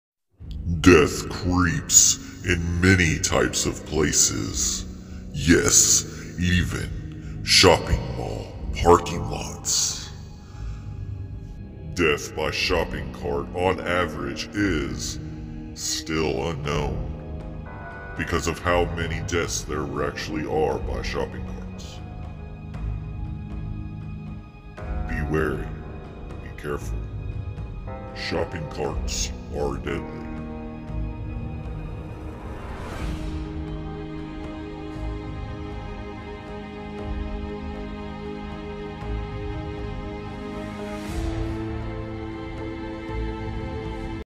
Shopping Cart Warning
A funny little video of movie props and voice over about death by shopping cart. tip at the end